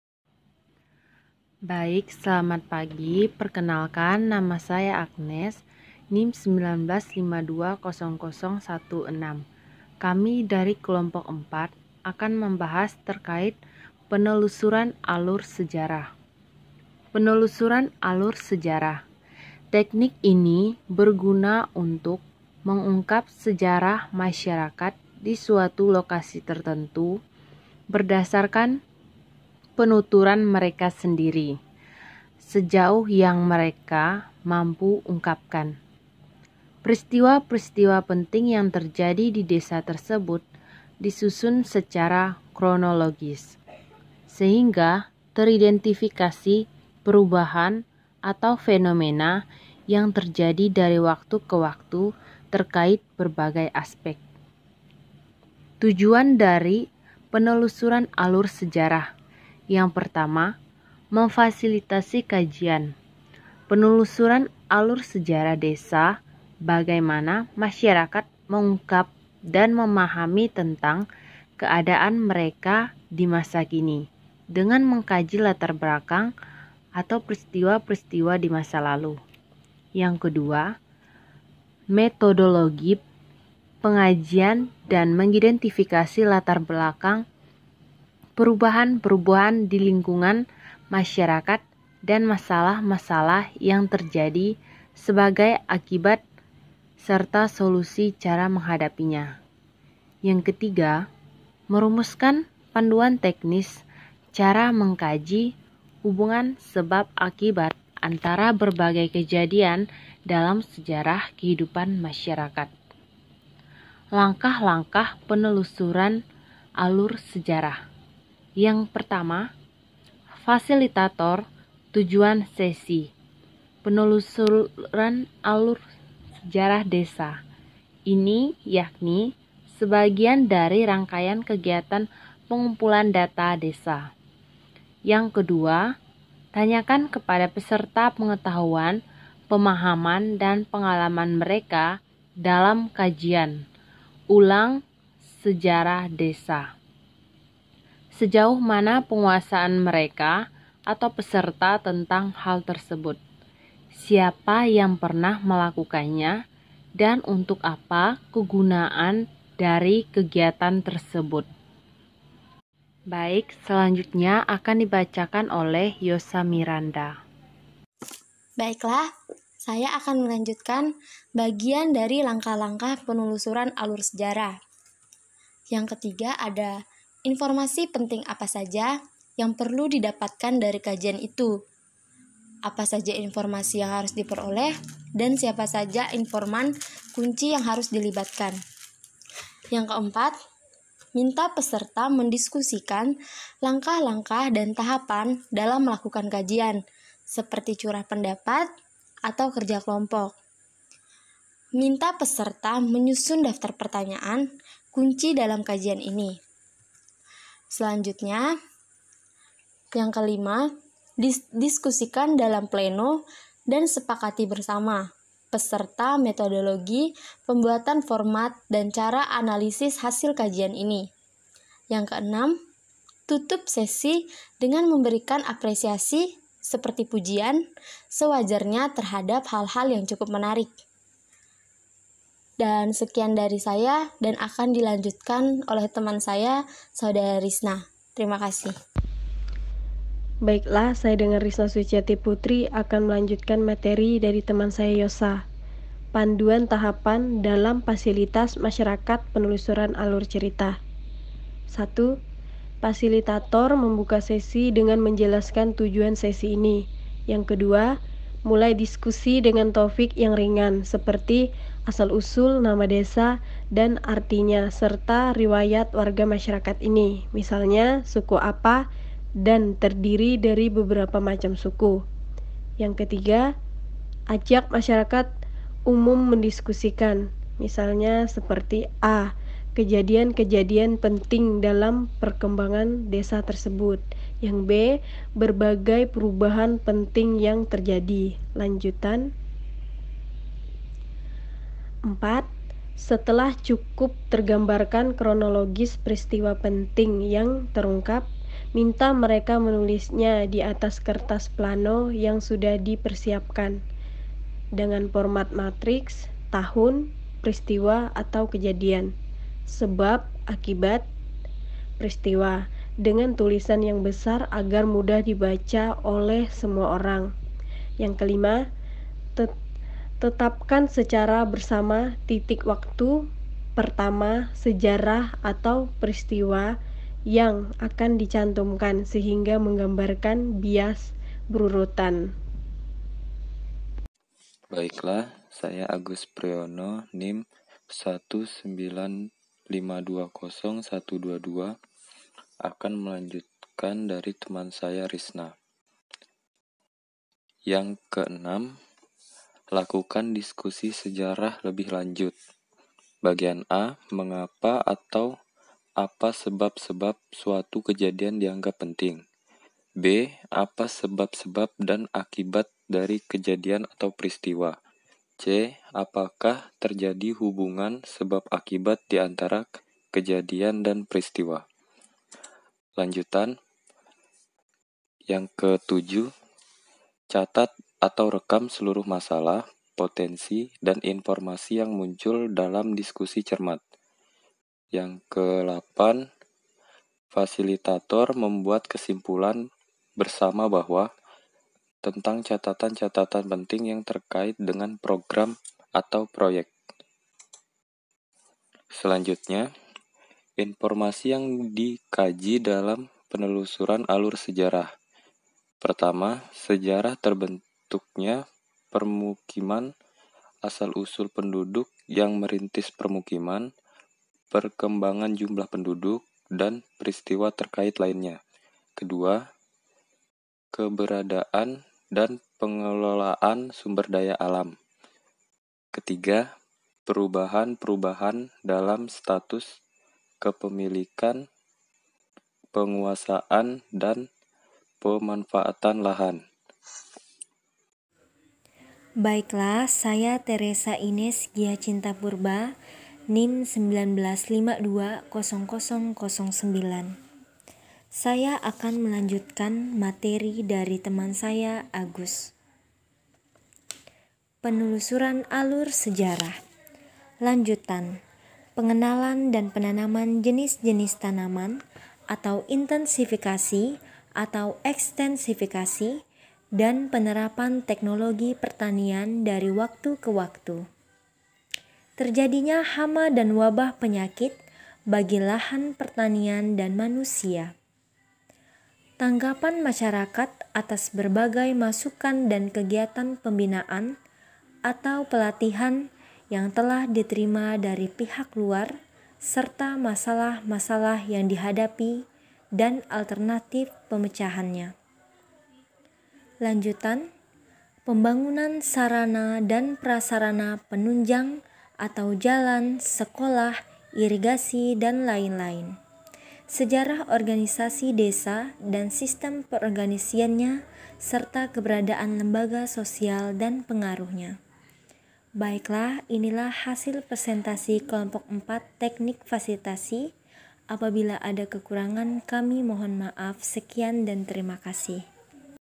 Karya Mahasiswa | Metode Fasilitasi | Penelusuran Alur Sejarah
Video ini dibuat oleh mahasiswa untuk memenuhi tugas kelompok matakuliah Tenik Fasilitasi Ilmu Pemerintahan Sekolah Tinggi Pembangunan Masyarakat Desa "APMD" Yogyakarta.

Video ini merupakan karya:
1. Yosa Miranda
2. Agnes
3. Theresia Ines Giacita Purba
4. Risna Suciati Putri
5. Agus Priono 💠💠💠
Perlengkapan Shooting Saya